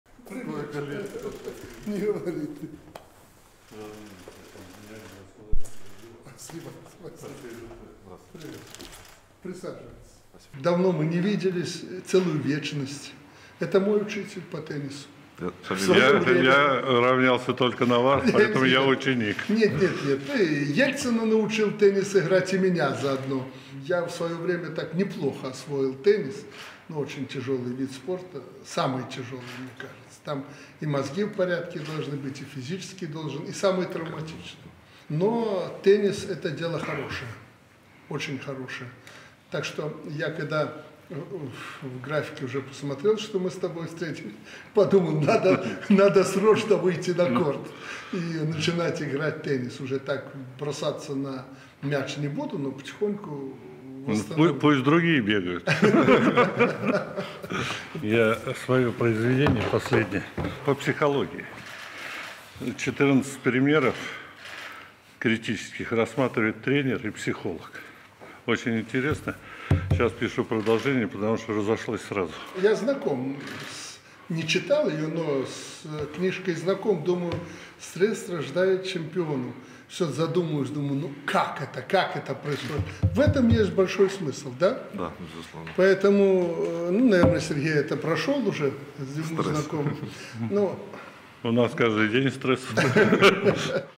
Лукашенко – Тарпищеву: Давно мы не виделись, целую вечность! Это мой учитель по теннису!
Лукашенко: Давно мы не виделись, целую вечность! Это мой учитель по теннису!

Президент Беларуси Александр Лукашенко 13 апреля встретился с президентом Федерации тенниса России Шамилем Тарпищевым.